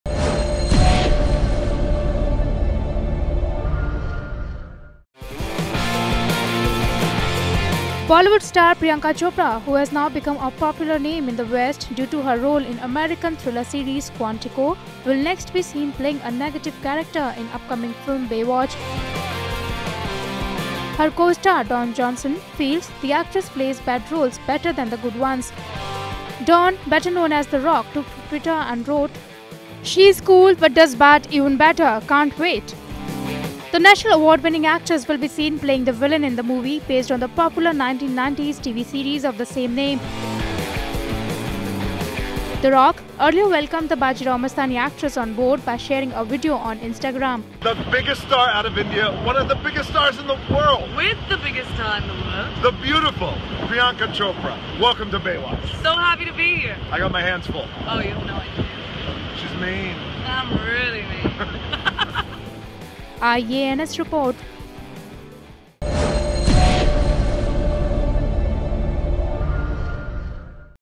The Rock Comments On Priyanka Chopra's acting | Baywatch
Dwayne Johnson, who will be sharing screen space with actress in Priyanka Chopra, feels the actress plays bad roles better than the good ones.